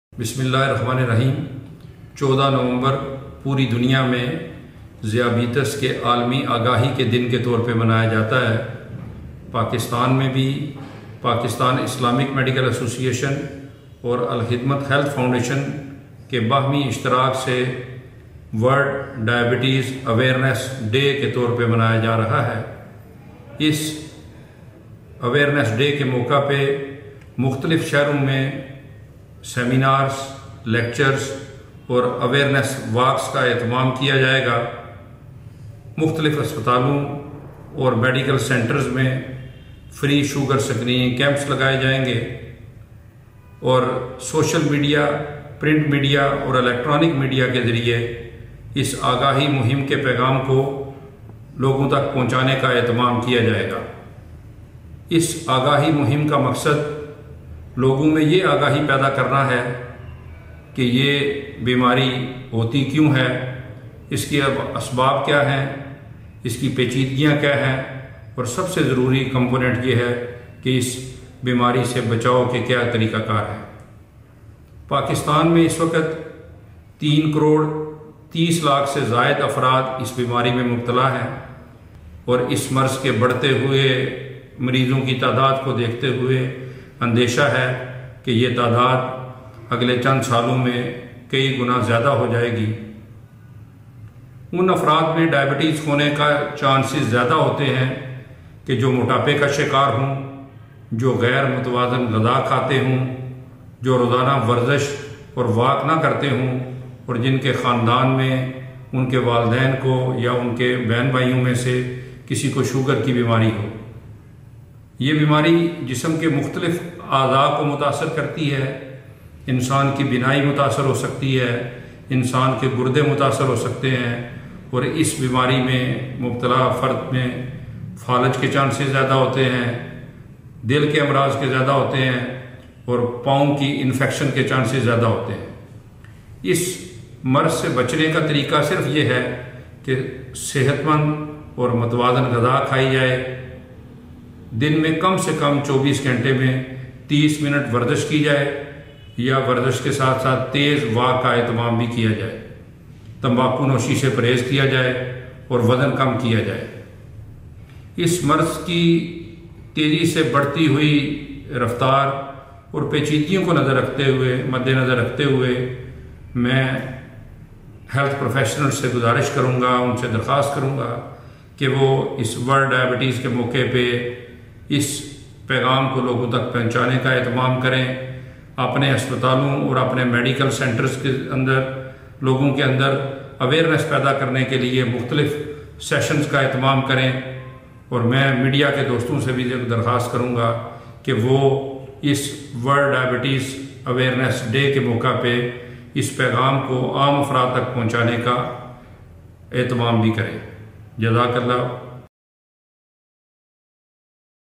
Rising Diabetes Risks: Our Role in Awareness and Prevention || Dr. Abdur Rehman
In this video, Dr. Abdul Rehman, Director of PIMA Relief Pakistan, discusses the increasing risks of diabetes in the country and emphasizes our responsibilities as healthcare professionals, individuals, and journalists in raising awareness.
Learn the basics of diabetes, including its symptoms, risk factors, and prevention.
Join PIMA Diabetes Awareness Campaign to spread knowledge on World Diabetes Day!